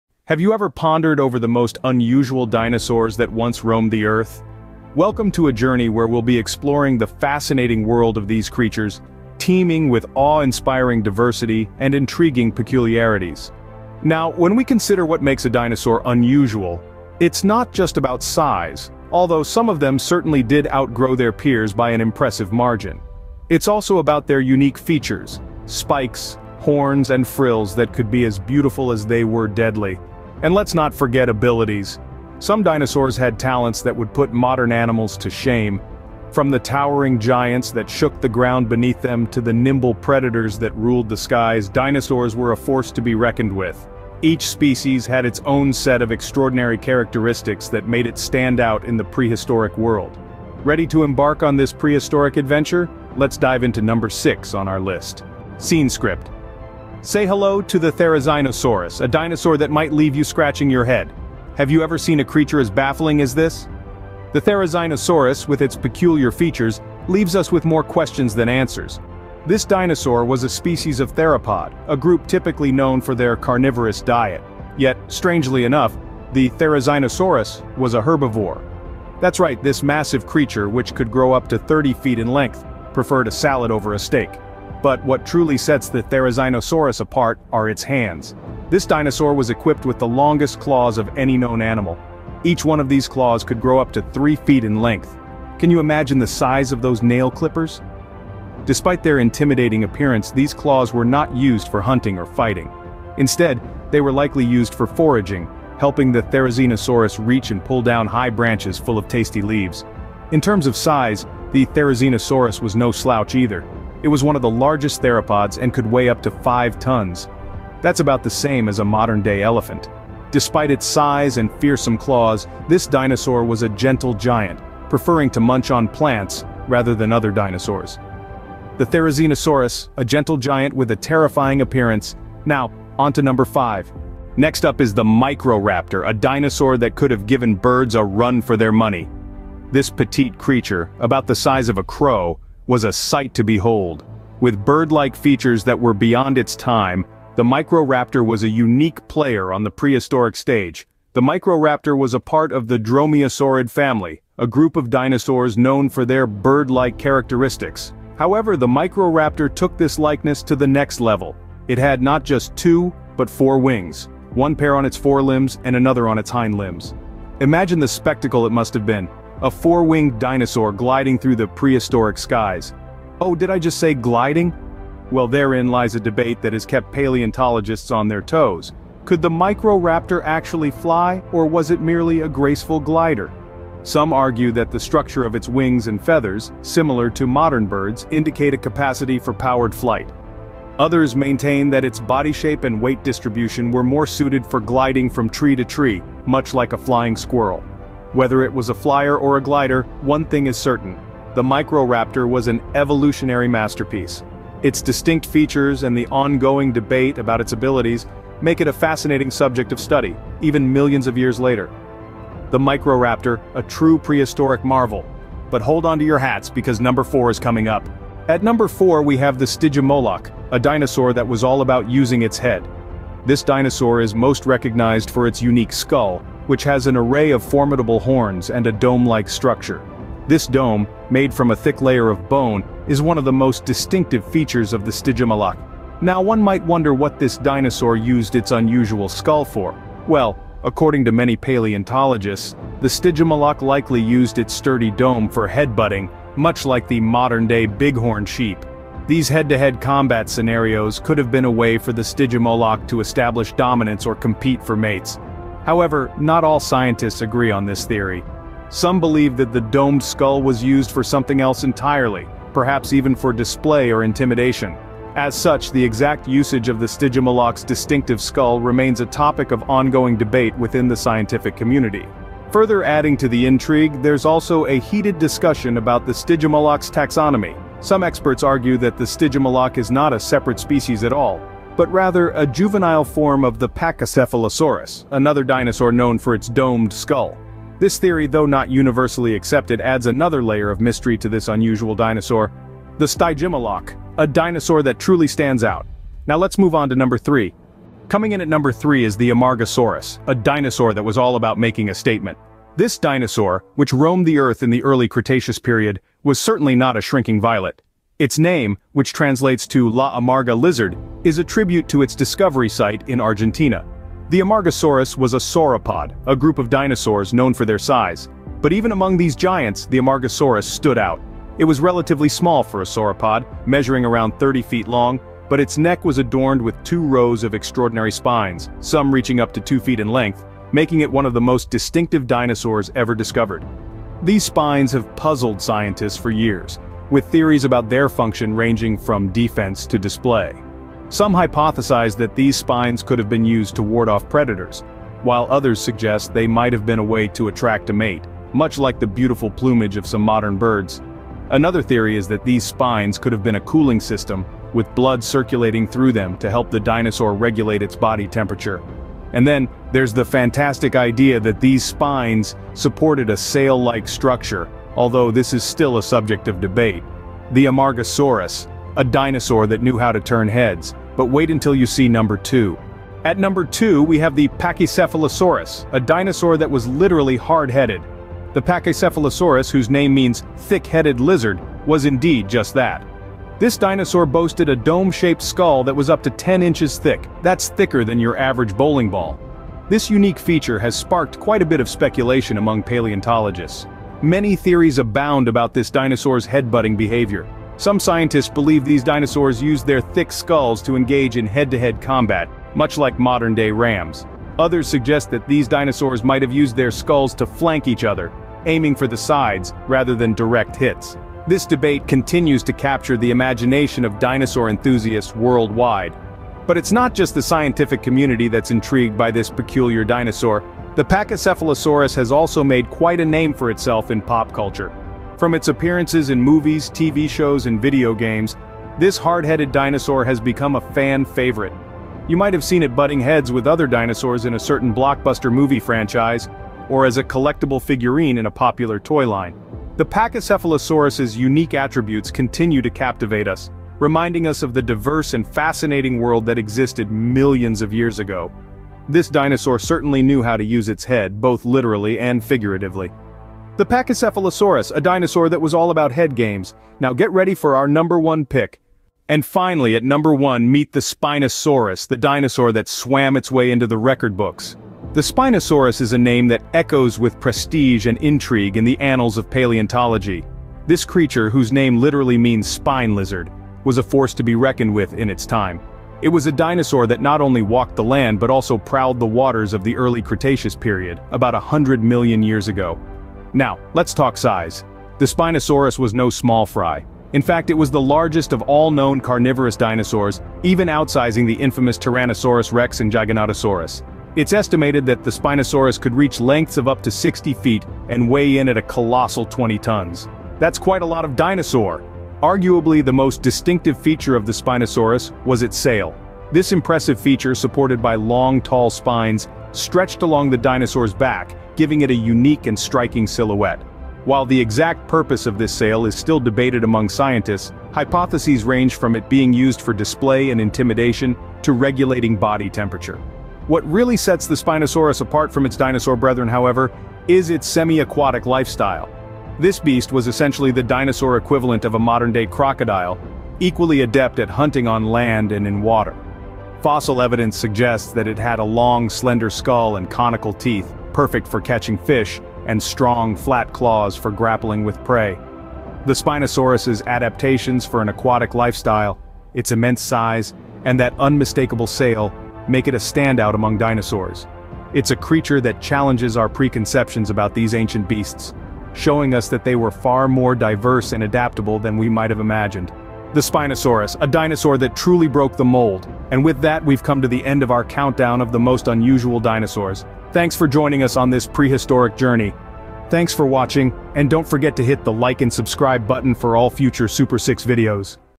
Top 6 Unusual Dinosaurs
Today we take a look at 6 of the more unusual dinosaurs that roamed the earth